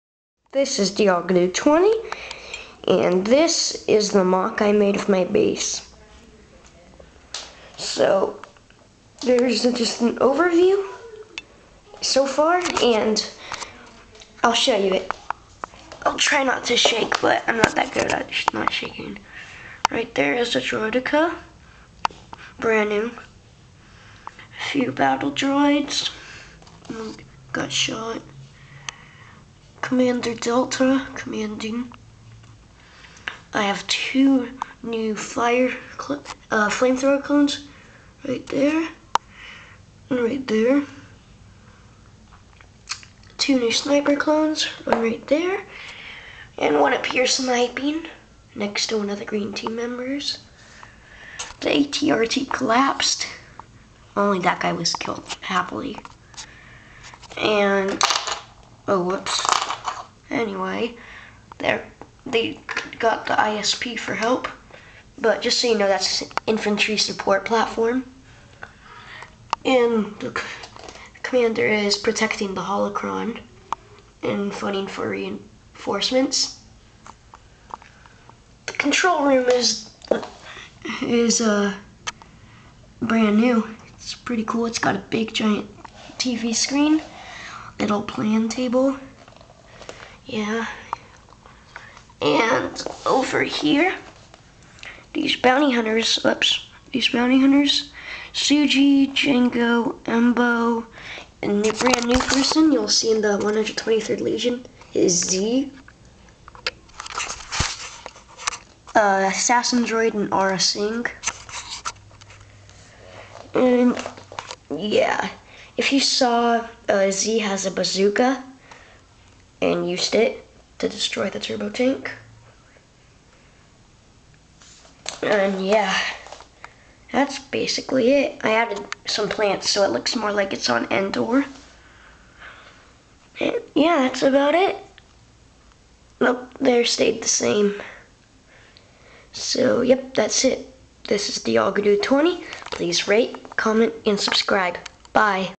My lego star wars MOC/Base on Endor (WIP)
upgraded now with a control room.